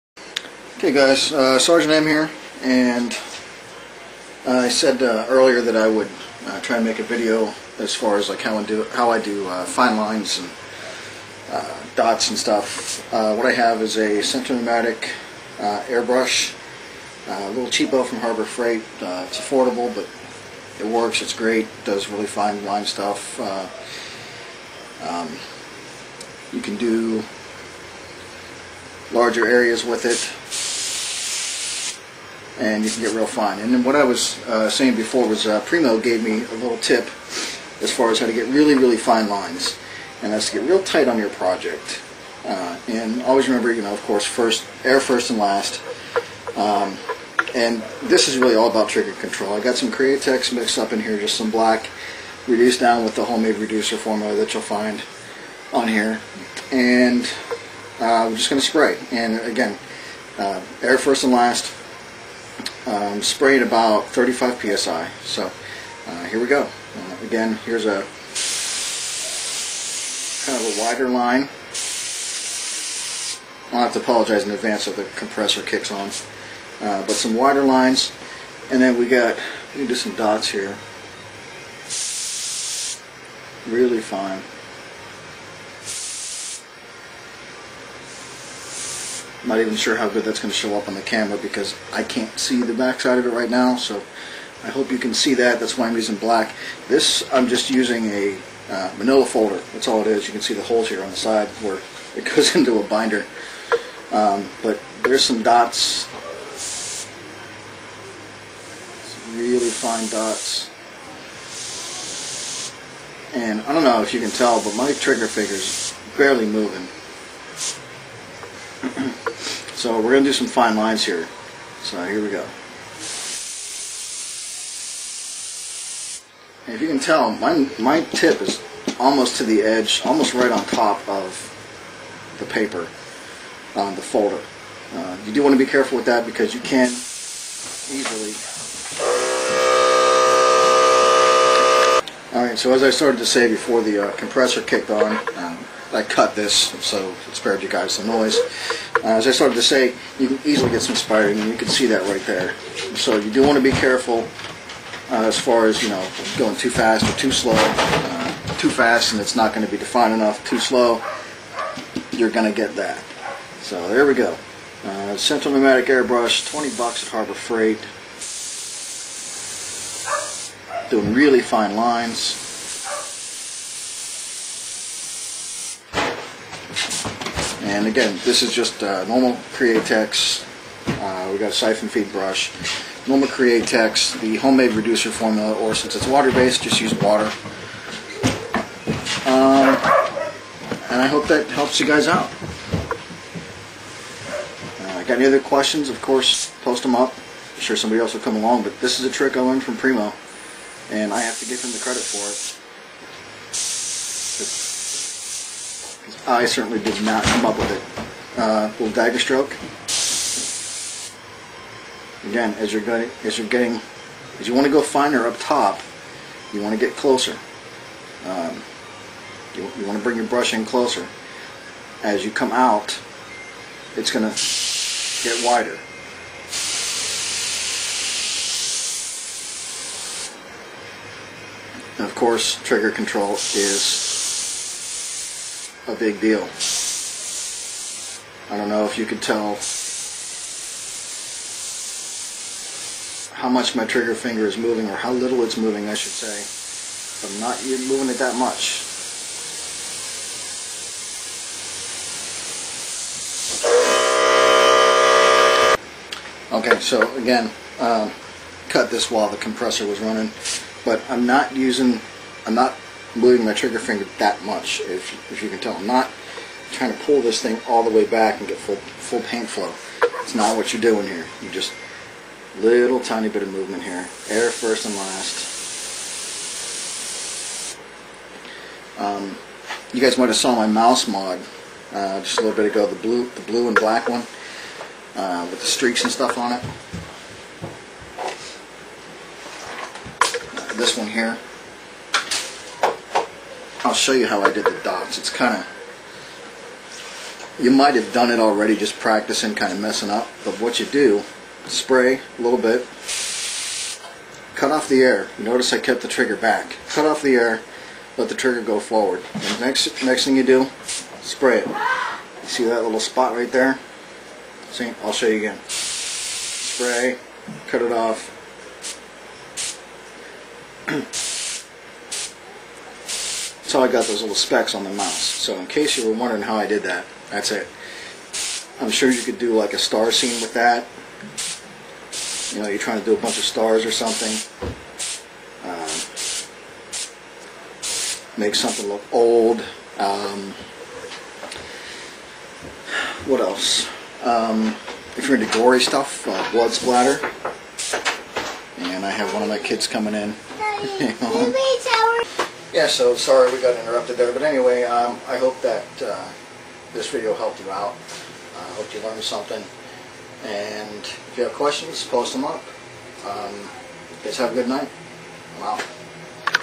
Airbrush Fine Lines and Dots
Airbrushing fine lines and dots with my Central Pneumatic airbrush from Harbor Freight.   No, it's not an expensive brush, and doesn't have a big name like Iwata or Dagr, but it does a really good job for being under $20.00.